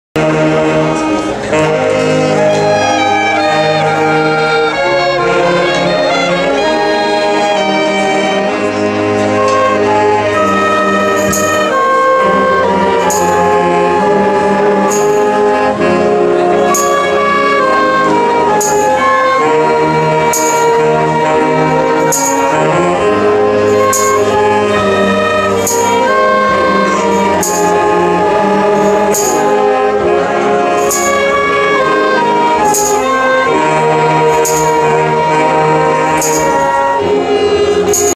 CM Lira N S do Amparo em Santo Antônio do Grama
3º Encontro de bandas de música em 23 de agosto de 2014.